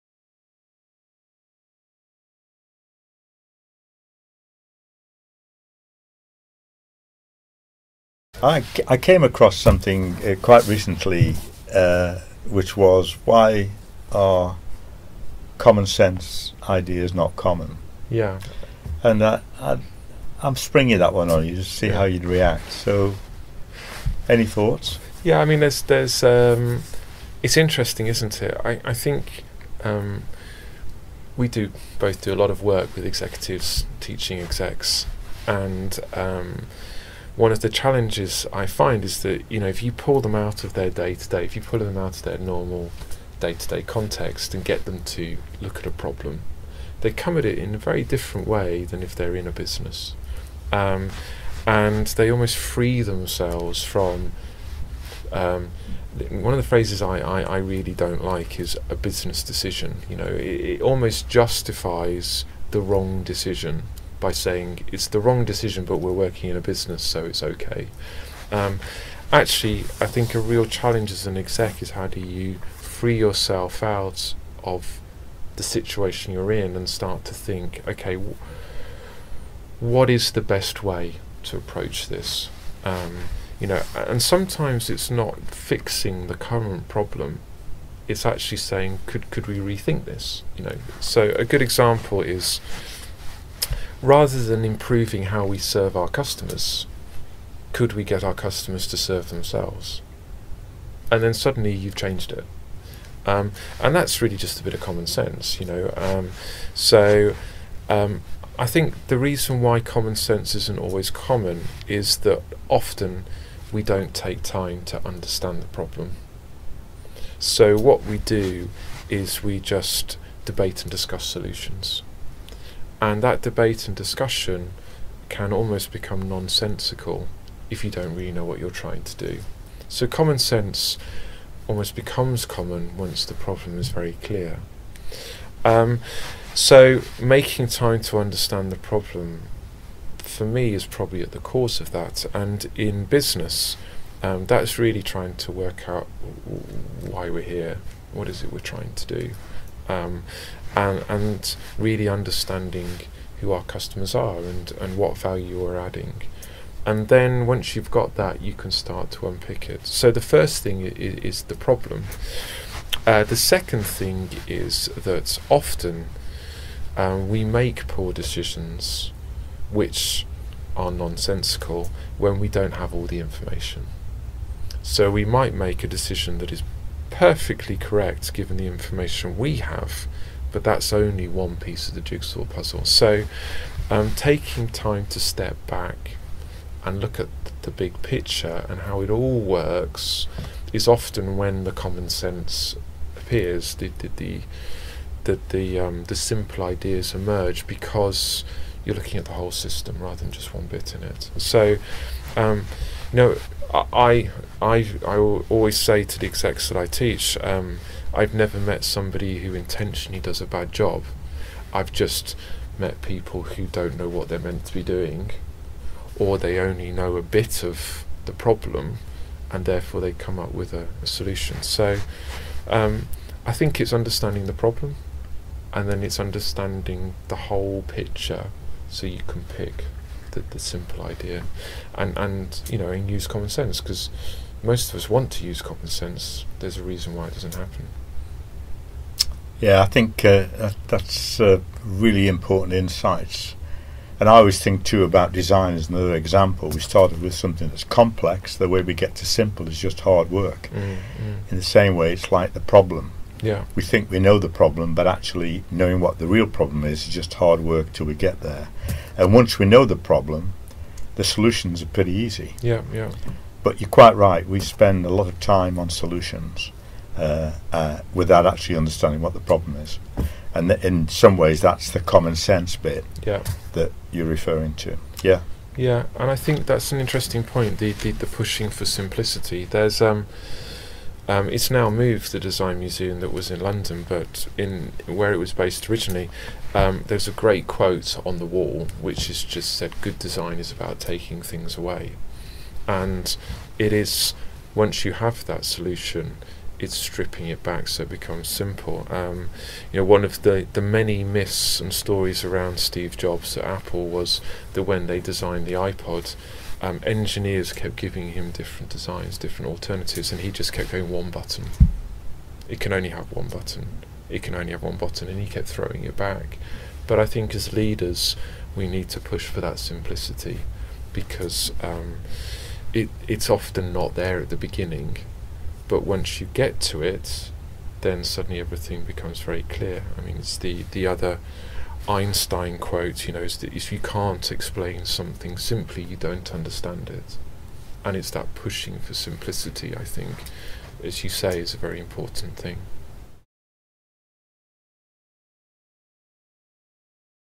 Common sense - Essential Operations Management
Alex and Terry Hill discuss the role of common sense, covering material in their book, Essential Operations Management, 2nd Edition.